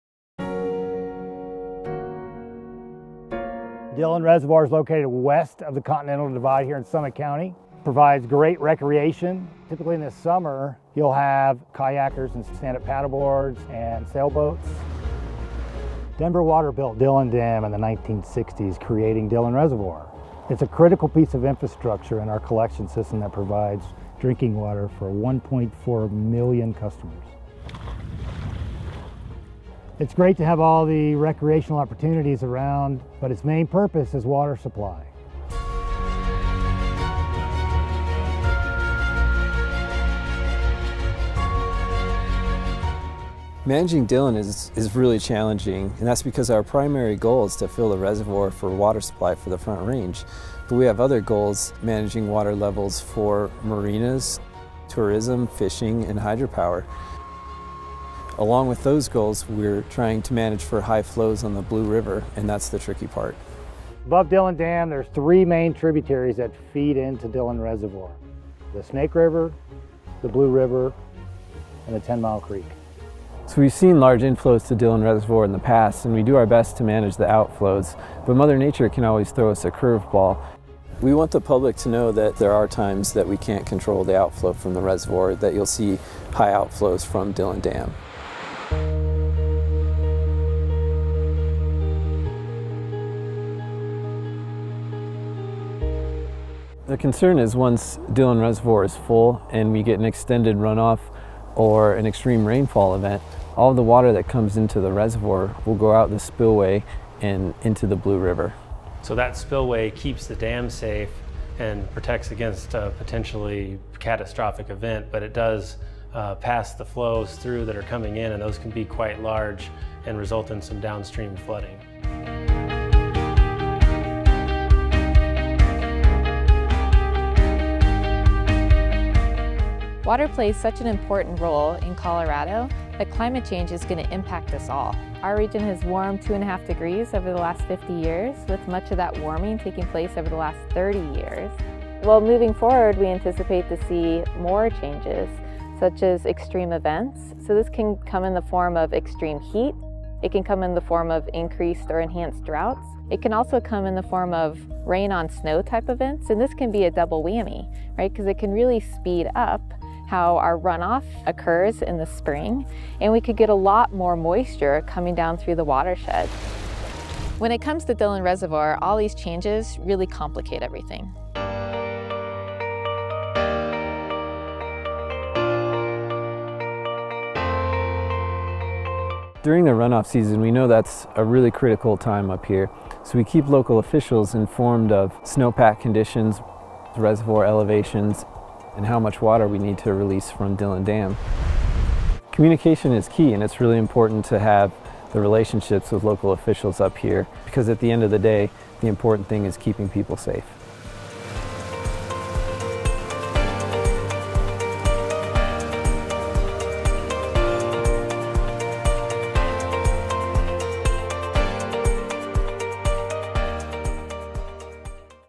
Dillon Reservoir's impact on runoff season
Denver Water owns and operates Dillon Reservoir in Summit County, Colorado. The reservoir's main purpose is to store water for Denver Water customers, but it has an impact on water levels below the dam on the Blue River. Learn about how the dam operates and the impact on flooding during the spring runoff.